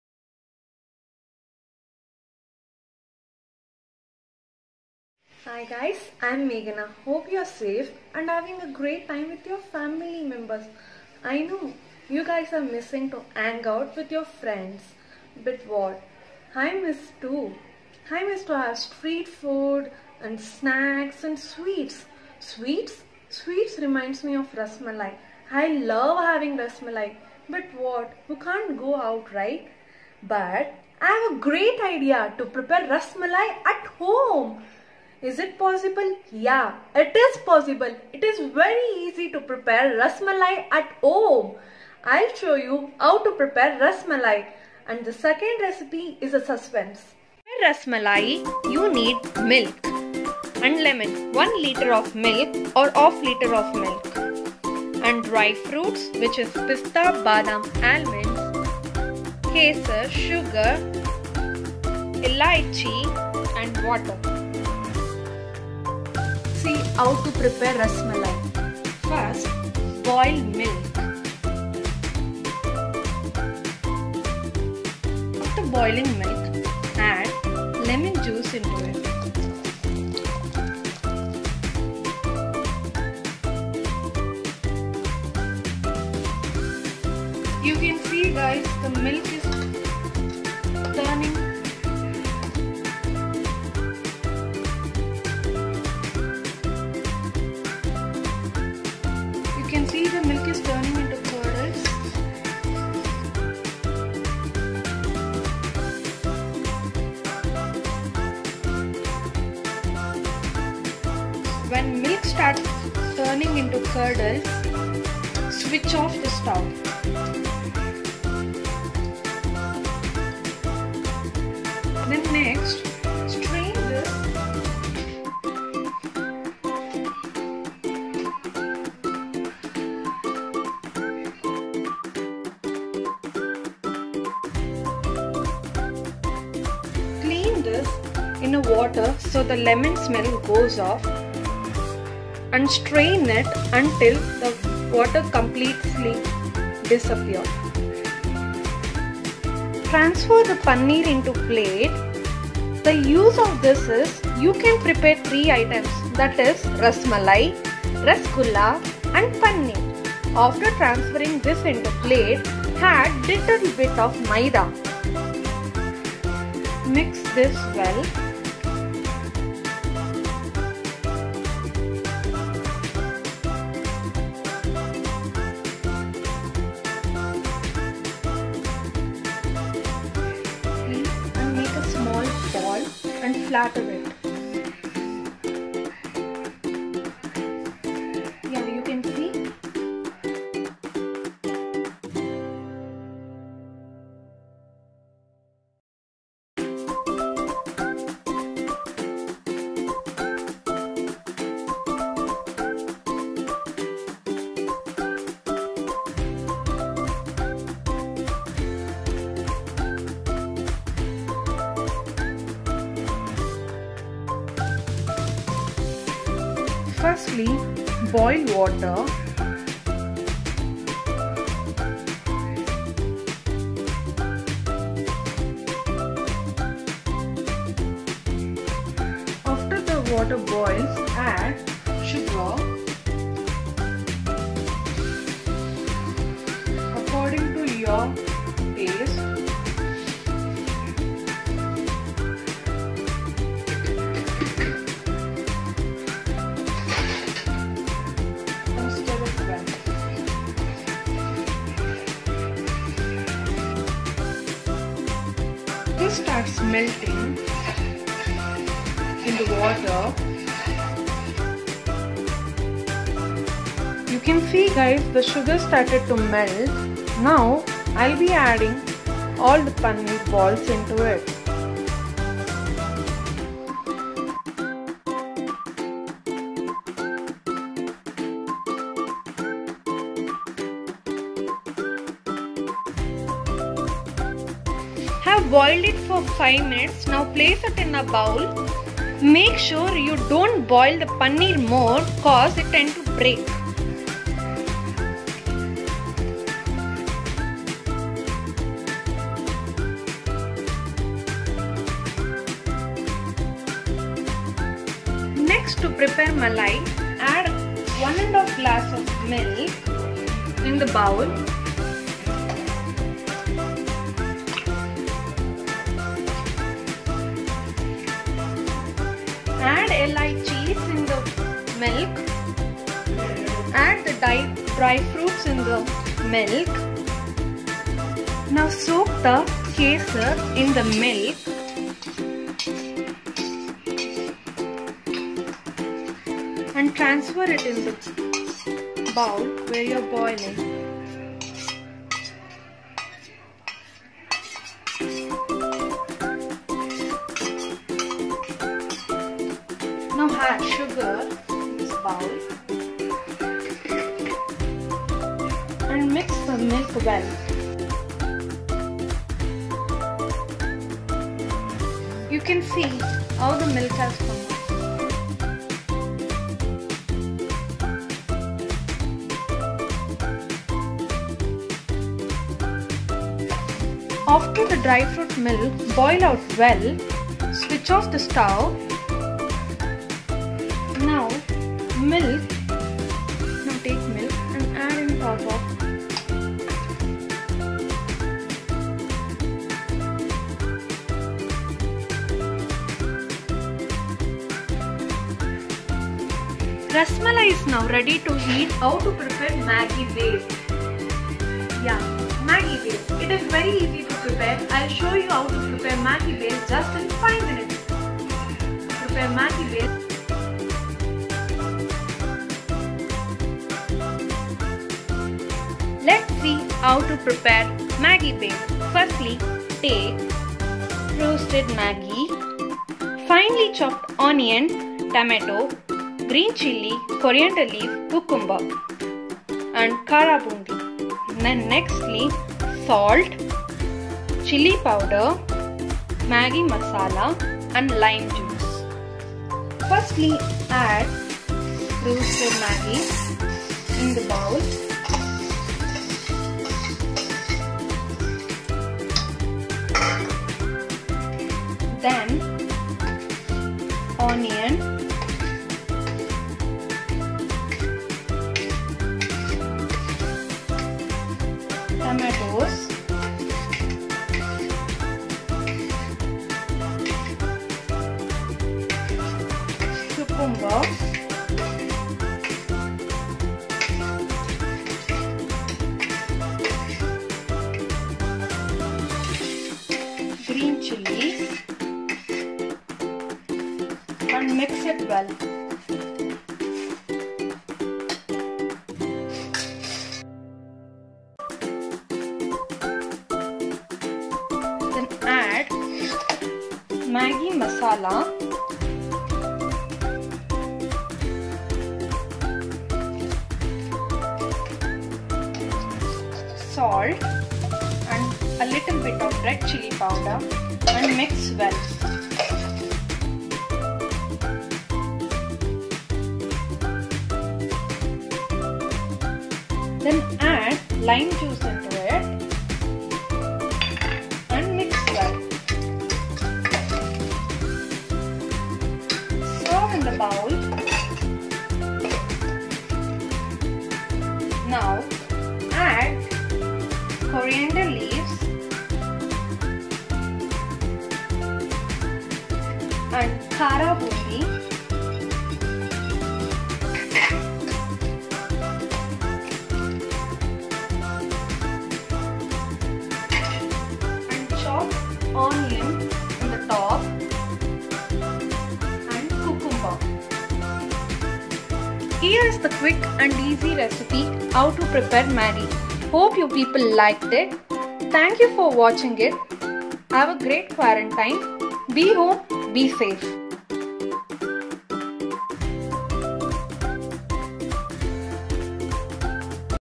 THE QUARANTINE SHOW (EPISODE 5 ) :- " SUGAR & SPICE "
Presenting you the Fifth Episode of "THE QUARANTINE SHOW -- SUGAR & SPICE "
Here Meghana G Krishna of 4th Sem BBA from Surana College will show you how 2 very distinct dishes can be prepared -- THE ROSHOMALAI & MAGGI BHEL !! One dish is a loved SUGARY indian dessert while the other is a SPICY Indian-Chinese fusion Chaat !!
One thing is for sure you will be left with a strong Midnight Craving if u watch this video in the night. .
Well Sorry & You are Welcome too...
Learn something new guys, keep smiling...